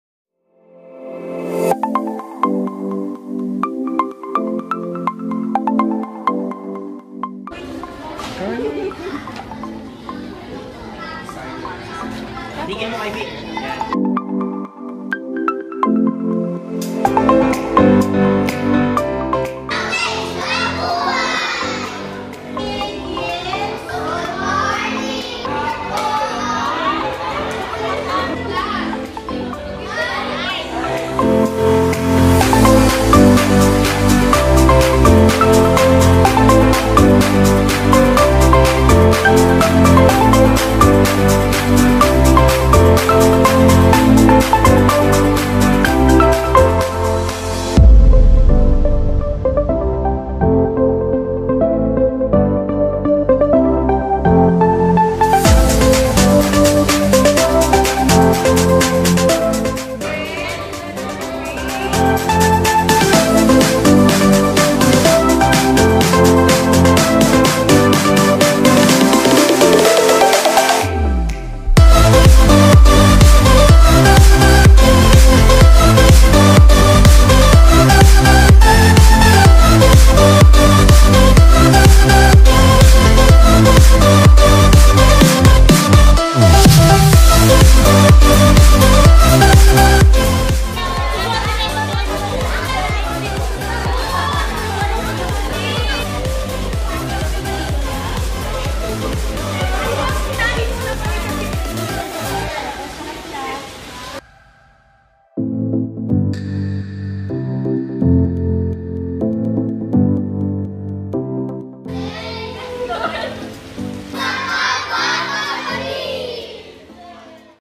iVolunteer Team visits Tulong Sa Kapwa Kapatid
The adventures of iVolunteer Team as we visited the community of Tulong Sa Kapwa Kapatid in Smokey Mountain, Manila

This is our quarterly volunteer day for the staff of iVolunteer to meet our partners, communities and volunteers we support.

to volunteer.